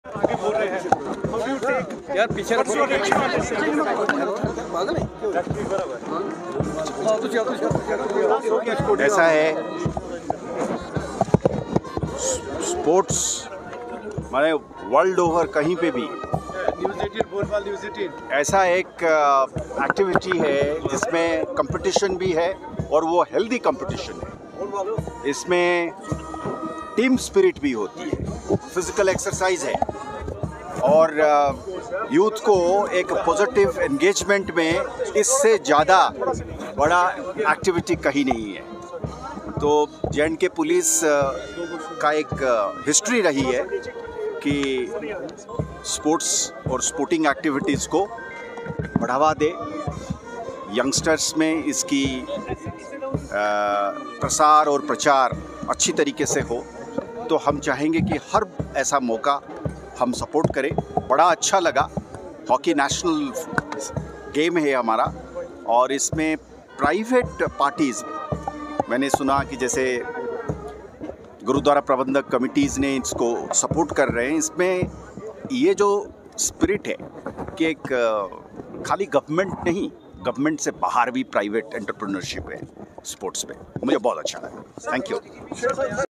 Sports is a healthy competition with team spirit and physical exercise, vital for youth engagement
Sports is a healthy competition with team spirit and physical exercise, vital for youth engagement J&K Police has a history of promoting sports, and we support such opportunities. It’s great to see private parties and the Gurudwara committee supporting this Hockey    national game, showing private entrepreneurship in sports: DGP J&K RRSwain.